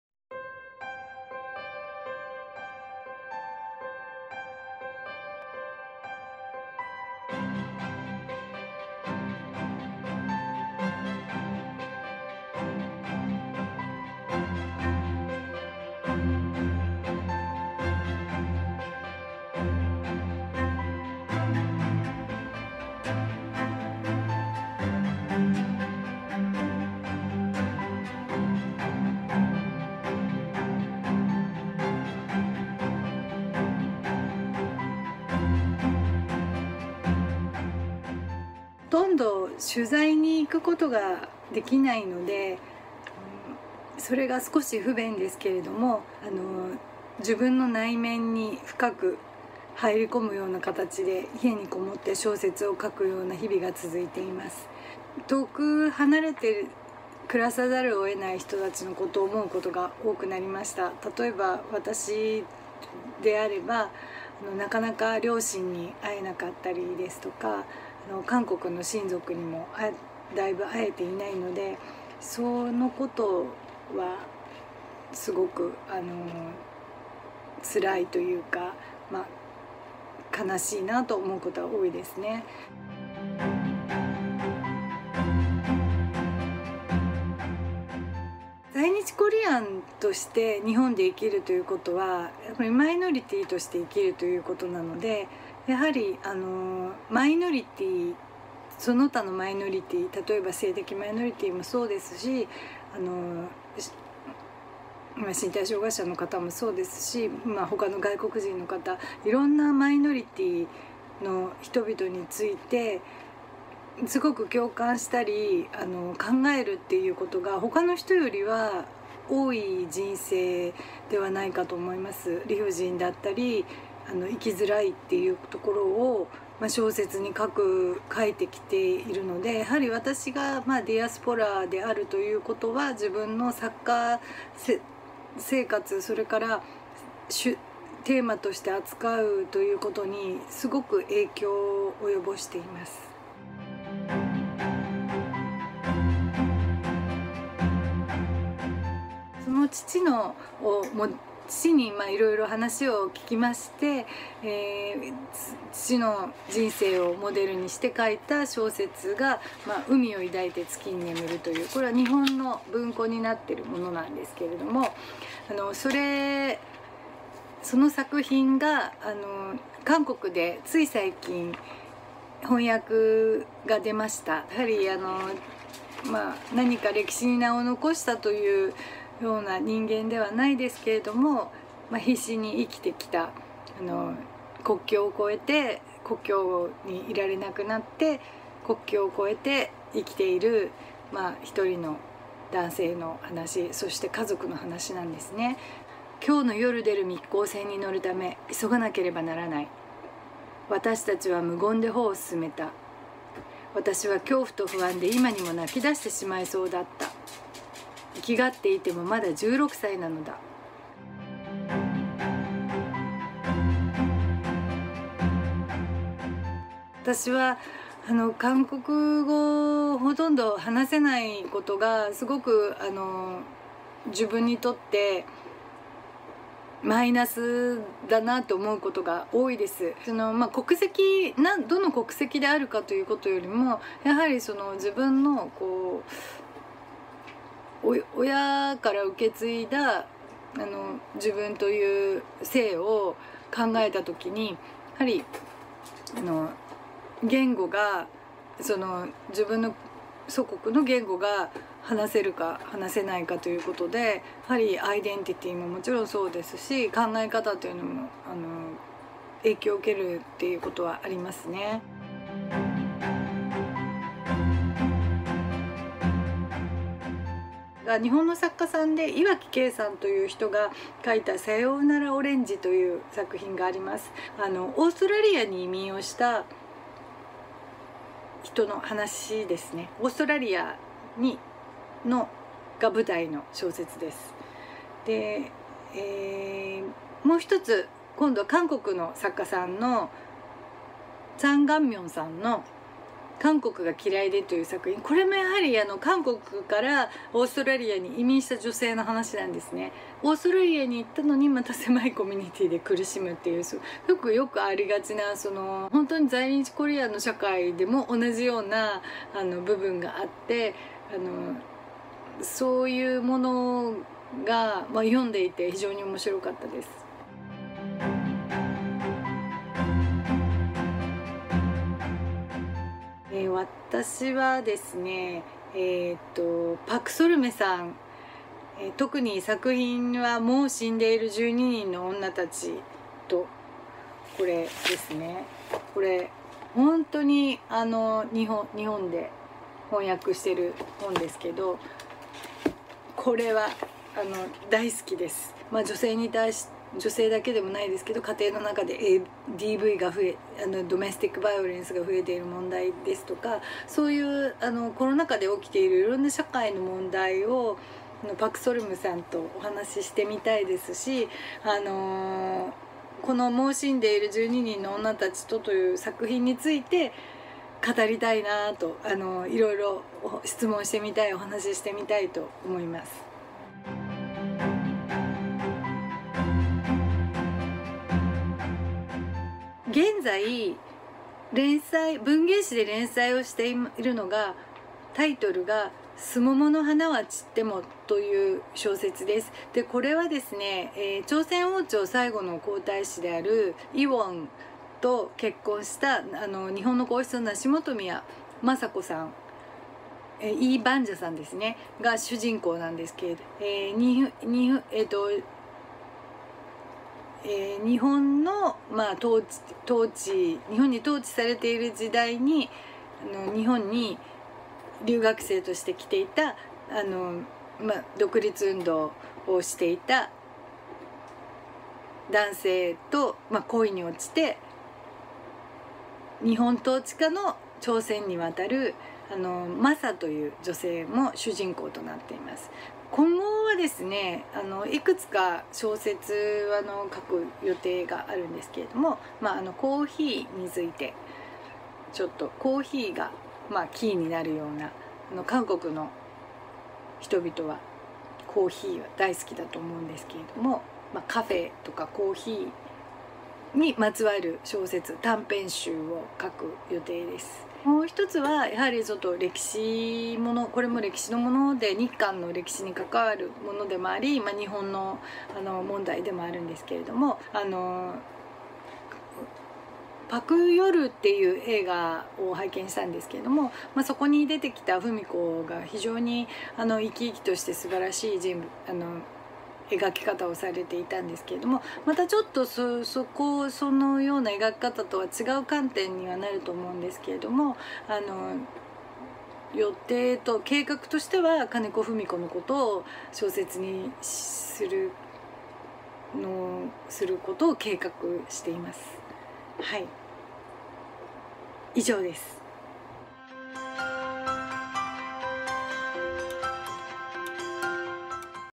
[2021 문학주간] 협력프로그램: '한국문학번역원' 해외 한인작가 9인 릴레이 인터뷰 3편 │후카자와 우시오
세계 곳곳의 한인작가 9인이 코로나 시대의 문학 활동, 디아스포라적 삶과 경험, 한국 문학 및 작가들에 대한 생각과 앞으로의 활동 계획에 대하여 이야기합니다.

3편의 주인공은 재일동포, 여성, 이민자의 삶을 주제로 일본에서 활발히 작품 활동 중인  재일교포 2세 작가 후카자와 우시오입니다.

국경을 넘어 나누는 우리 문학의 성취
10월 21일부터 매주 목요일 오후,
 2021 문학주간을 통해 만나보세요!